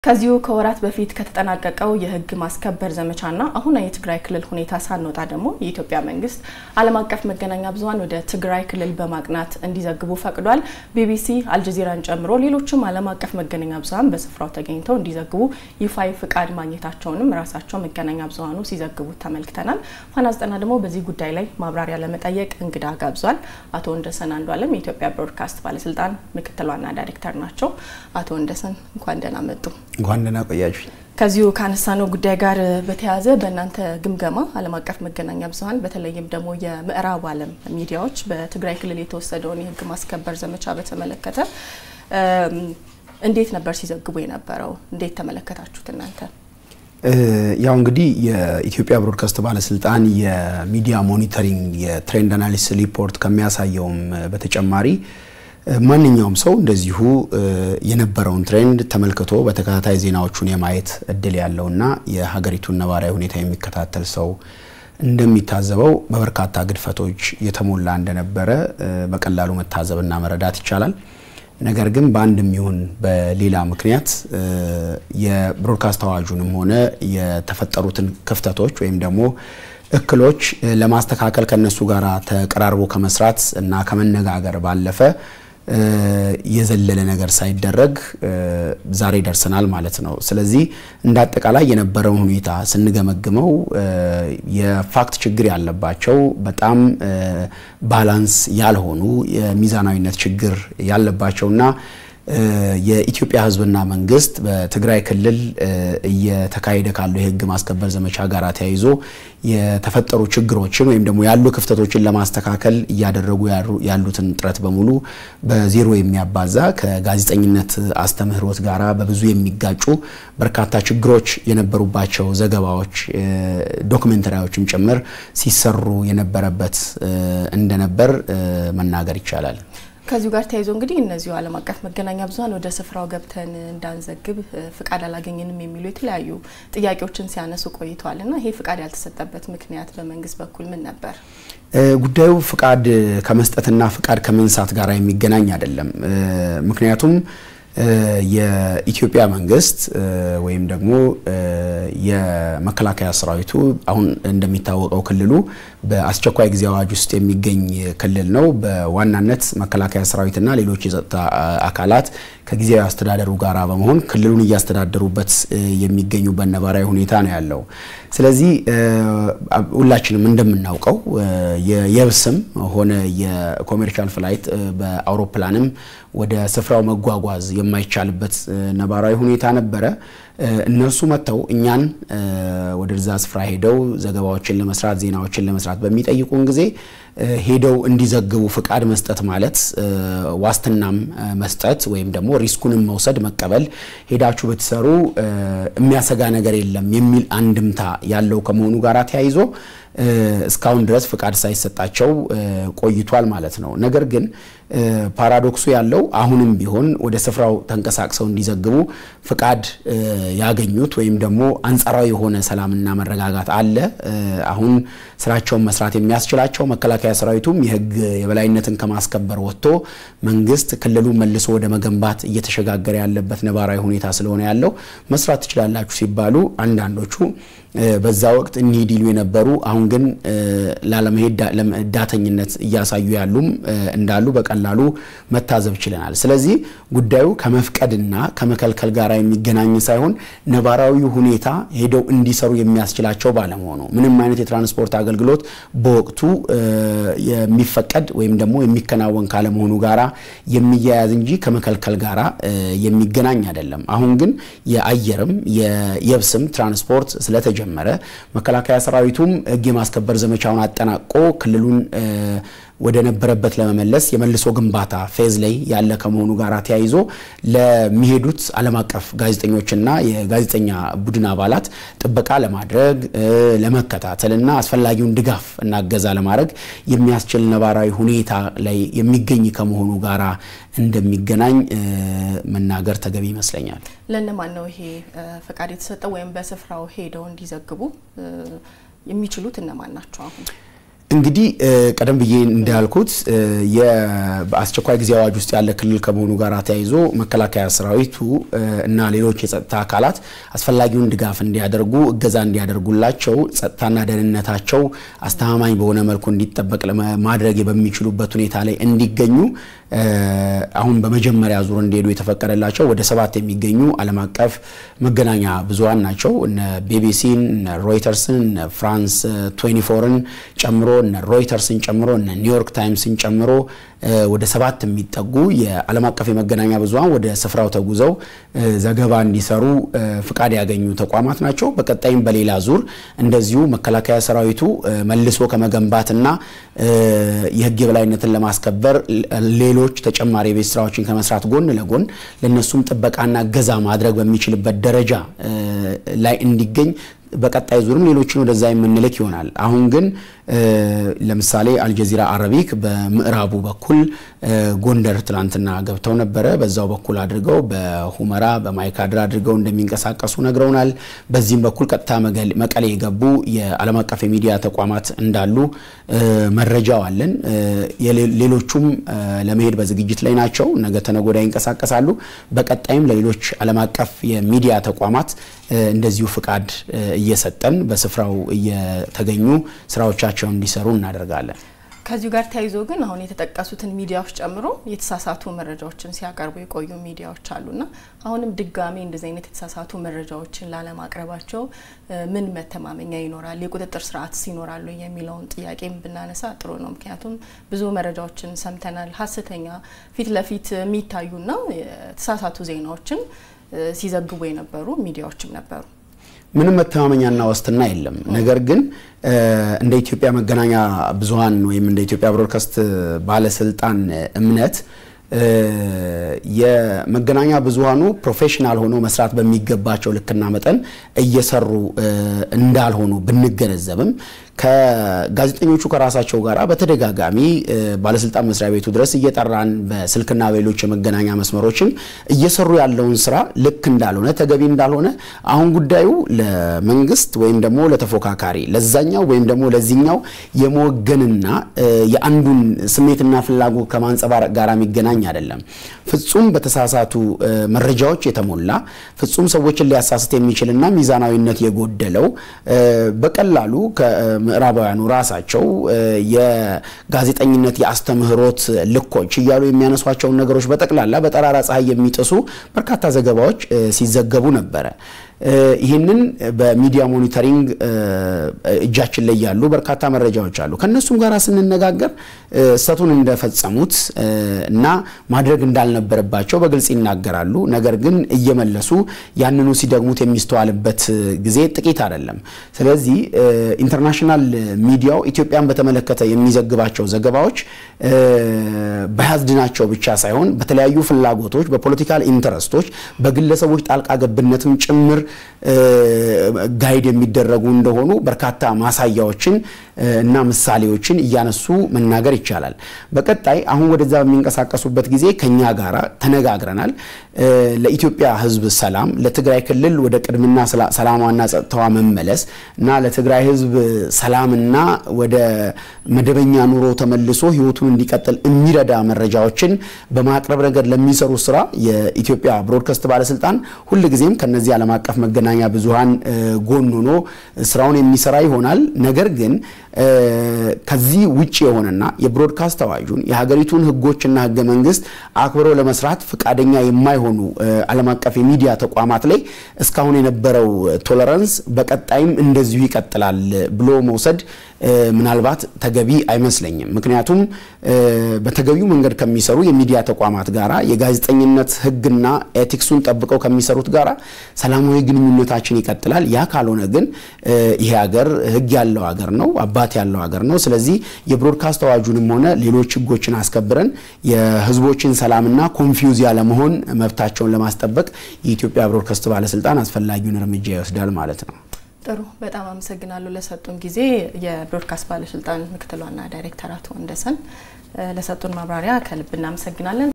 መንግስት ዓለም አቀፍ መገናኛ ብዙኀን ወደ ትግራይ ክልል በመግባት እንዲዘግቡ ፍቃድ መሰጠቱን አስመልክቶ የኢትዮጵያ ብሮድካስት ባለስልጣን ማብራሪያ
የኢትዮጵያ መንግስት አለም አቀፍ መገናኛ ብዙሃን ወደ ትግራይ ክልል በማቅናት አንዲዘግቡ ፈቅዷል።

ቢቢሲ፤ አልጀዚራን ጨምሮ ሌሎች አለም አቀፍ መገናኛ ብዙሃን በስፍራው ተገኝተው እንዲዘገቡ ይፋዊ ፈቃድ ማግኘታቸውንም ራሳቸው መገናኛ ብዙሃን ሲዘግቡት ተመልክተናል።